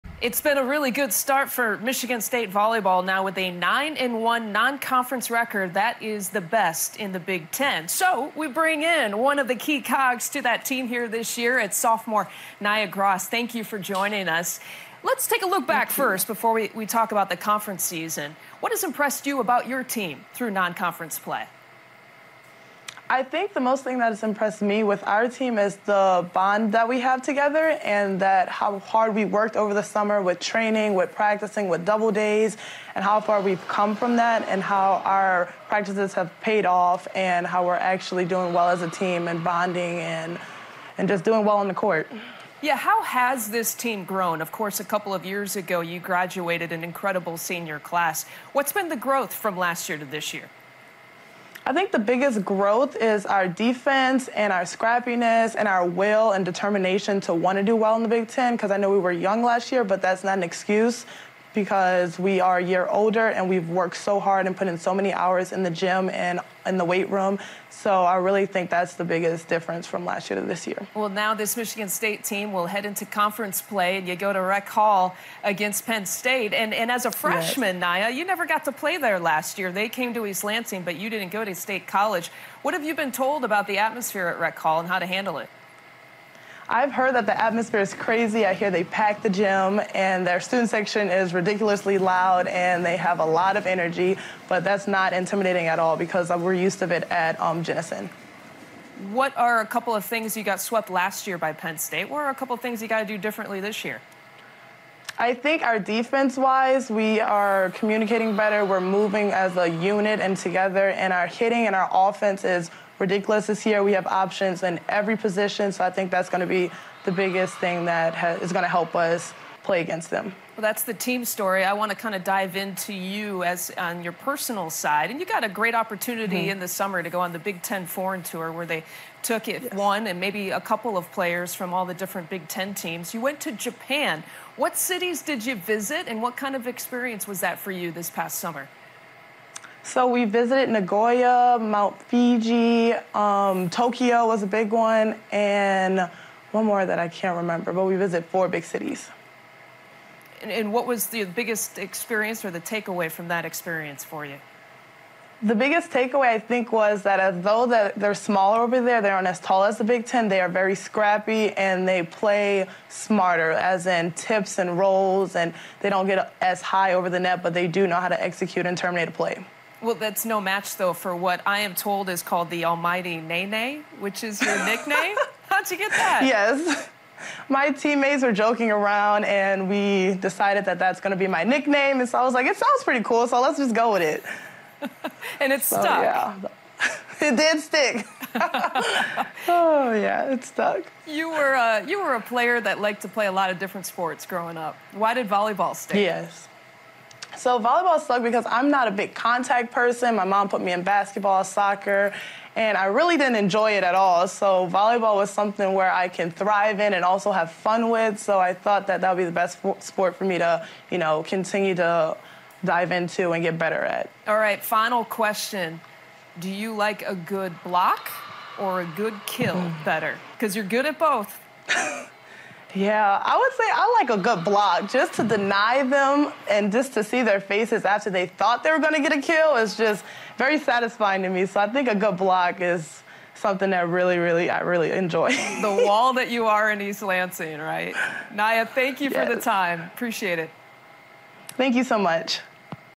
Naya Gros Talks Spartans Volleyball Success | B1G Volleyball
Michigan State's Naya Gros talks about the strong start to the season as the team heads into conference play.

Image courtesy of Michigan State Athletics.